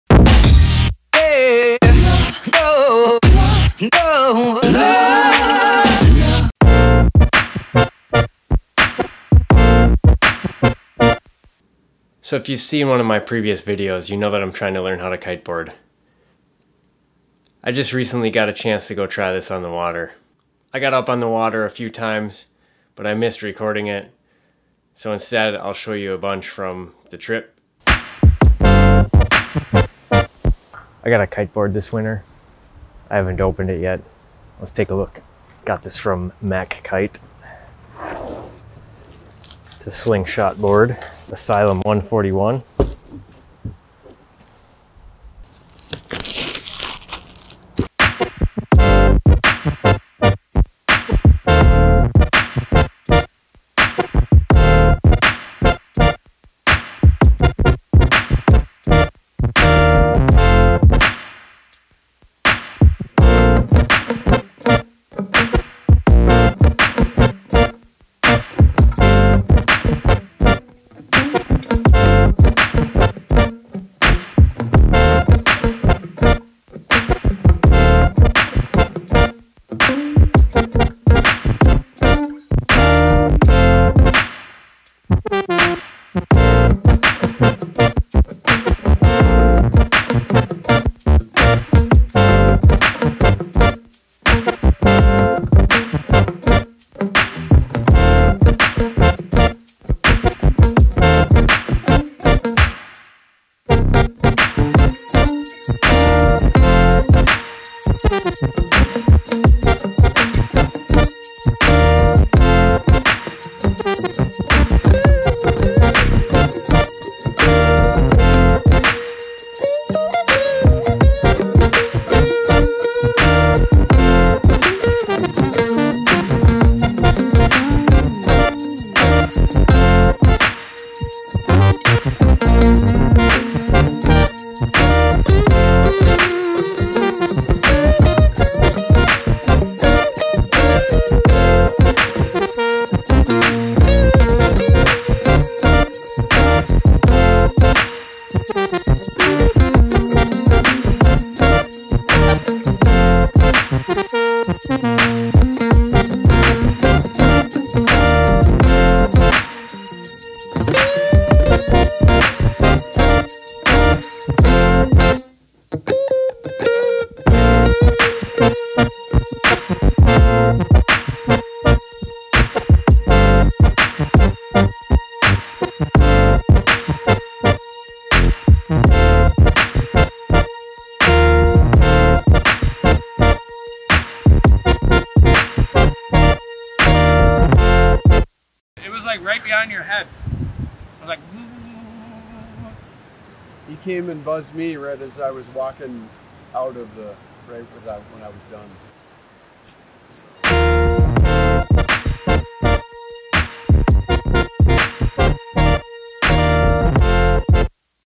Weekend Adventures S3E04 - Kite on the Water
Learning to Kiteboard on the Water at Lake Mille Lacs.

0:00 Intro
0:06 Tricks
0:11 Monologue
0:30 Unboxing new Kiteboard
1:06 Lake Mille Lacs
3:08 Wrap up